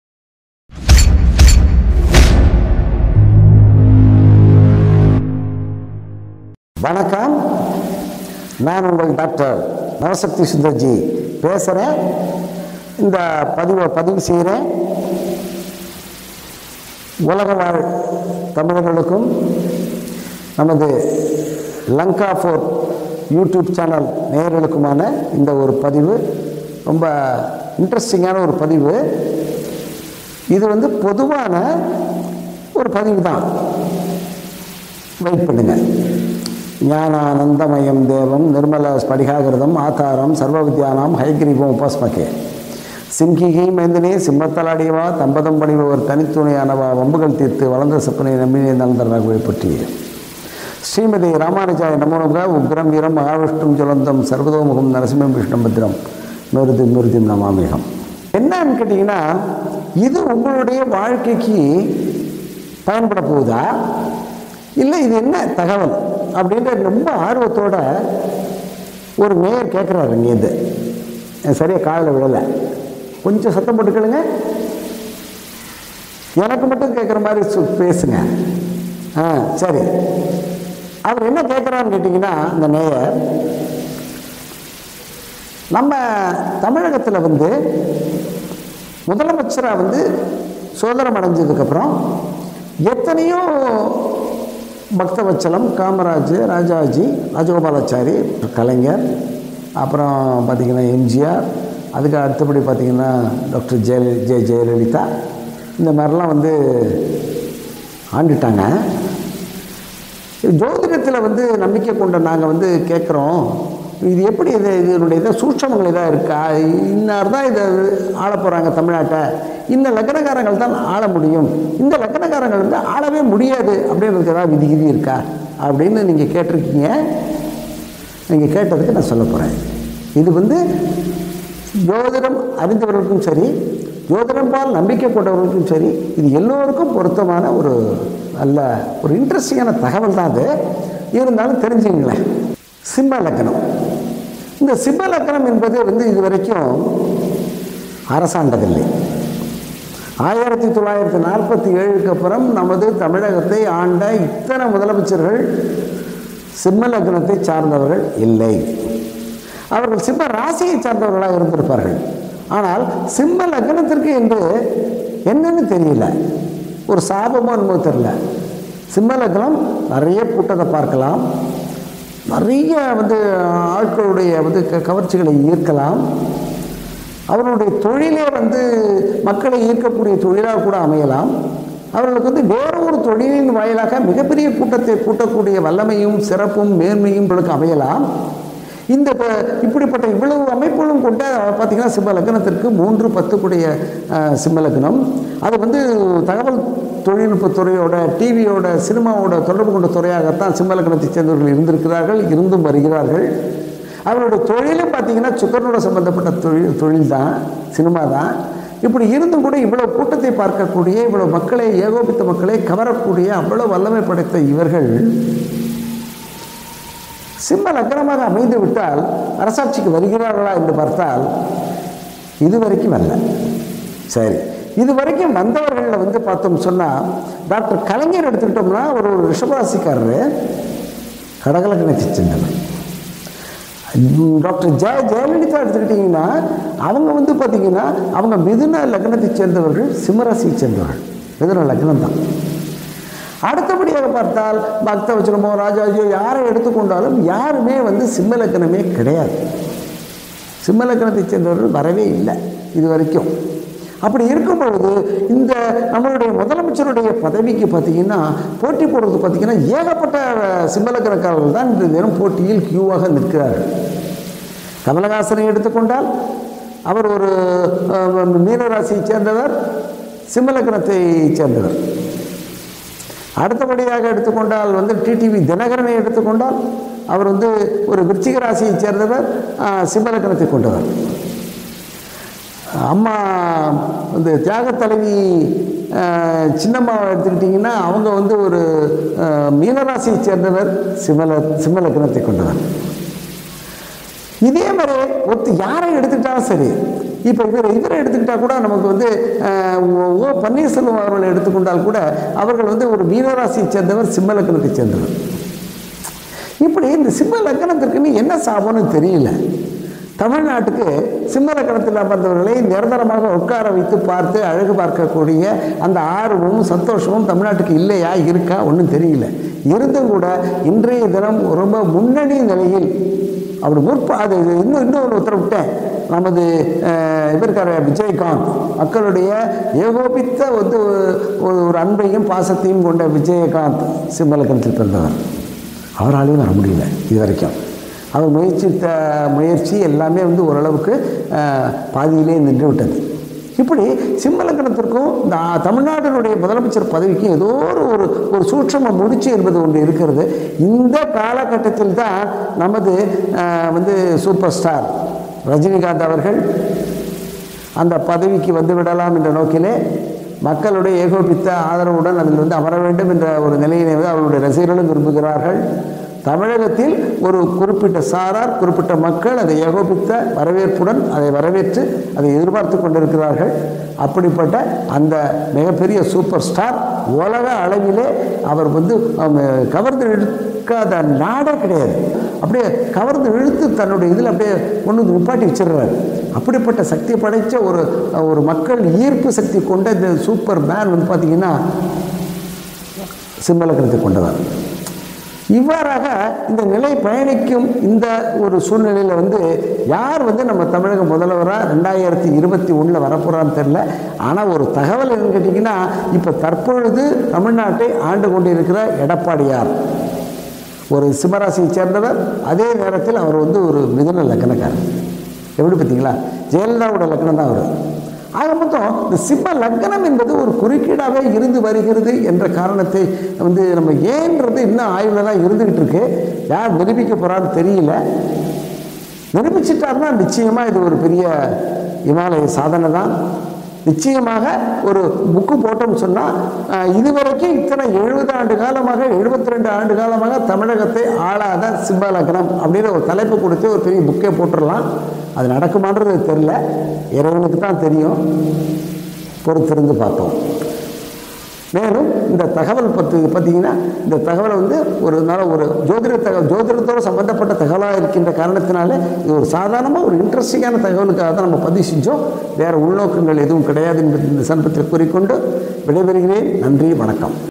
தமிழகத்தில் சிம்ம லக்கினக்கினகாரர்கள் அரசாள முடியாமல் போவது ஏன்? : டாக்டர் நவசக்தி சுந்தர்ஜி
தமிழகத்தில் சிம்ம லக்கினக்கினகாரர்கள் அரசாள முடியாமல் போவதன் இரகசியத்தை உடைக்கிறார் : டாக்டர் நவசக்தி சுந்தர்ஜி